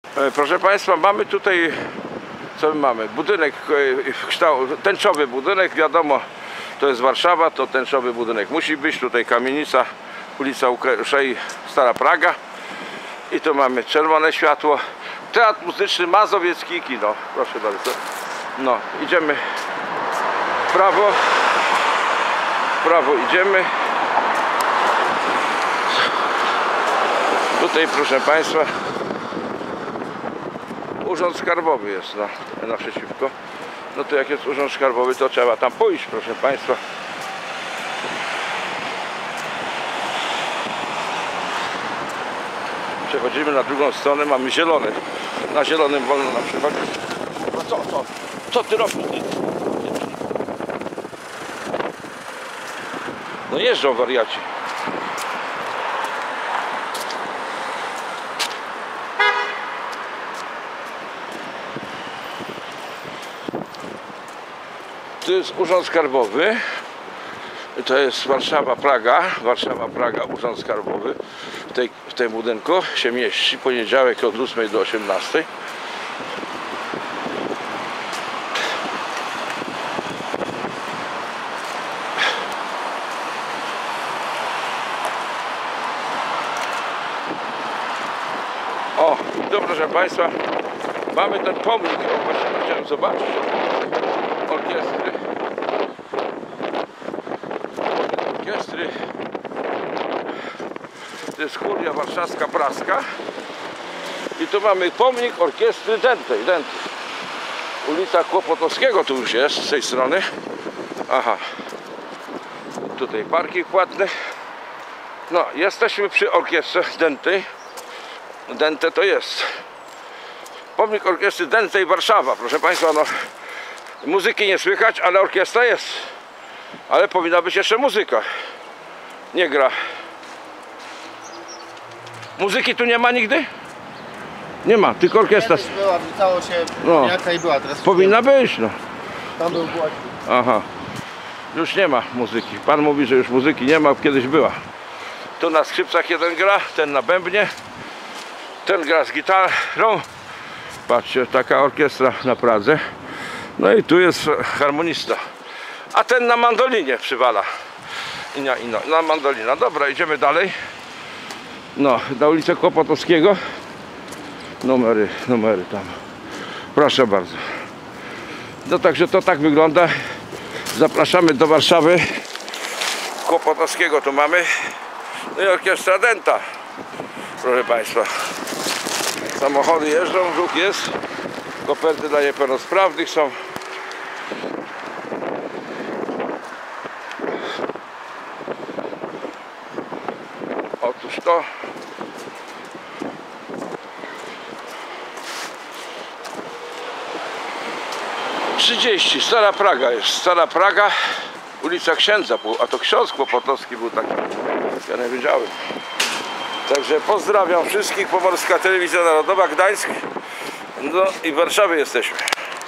WARSZAWA,TĘCZOWY URZĄD SKARBOWY JEST TAM A OBOK ...GRA ORKIESTRA PRASKA , TYLKO ALE JEST CICHO TAM?
Prezydent Biden przyjechał do Warszawy i dużo mówił o tym , że Ukraińcy muszą walczyć i NATO będzie ich wspierać .USA i ich sojusznicy będą dalej dostarczać broń dla Ukrainy. Wiemy jednak że jest to dolewanie oliwy do ognia i w efekcie końcowym może to spowodować rozszerzenie tego konfliktu na terytoria sąsiednich krajów Wszystkim nam obywatelom Rzeczpospolitej Polskiej zależy aby ta wojna szybko się zakończyła i zapanował na całym świecie pokój ! Dlaczego więc prezydent Biden i inni politycy nie czynią wszystkiego co należy aby POKÓJ nam zapanował!?